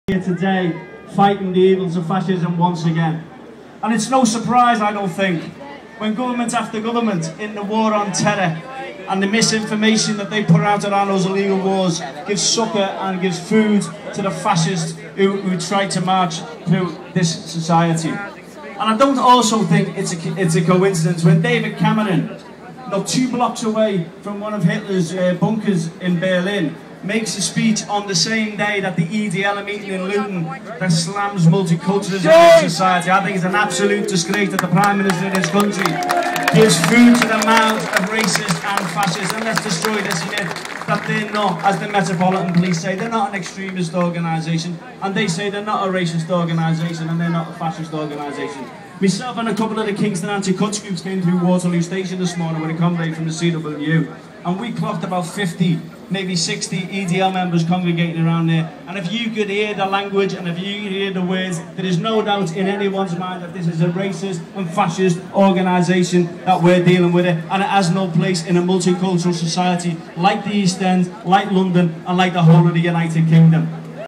UAF Anti EDL protest Whitechapel London 3rd Sep
follow @Kurdishblogger on twitter
United Against Fascism (UAF) Anti English Defence League (EDL) protest in  East London.

Hundreds of Muslims, Christians, Jewish people, atheists, gay and lesbians came together on the streets of the East End to show their unity against EDL and fascism.